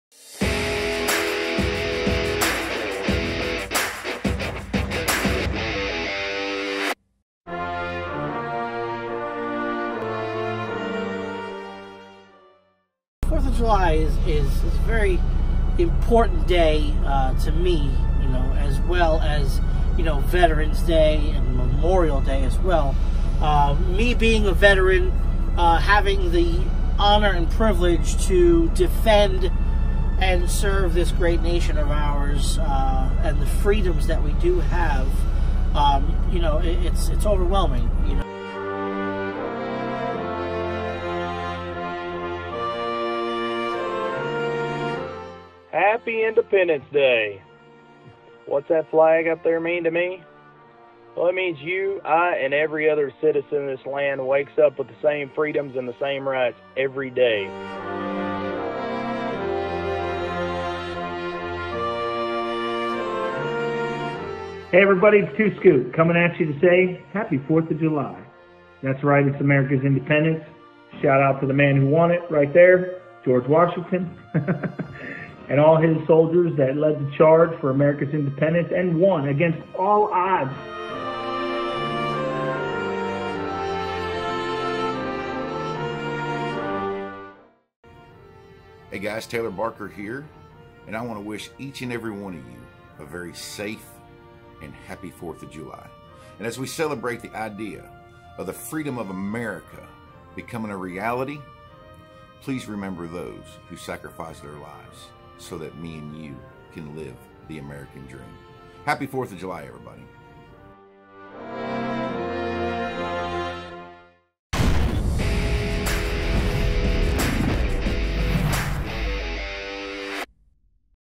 4th of July Tribute
A very special 4th of July tribute to all of the Great American Truckers out there from some of our friends.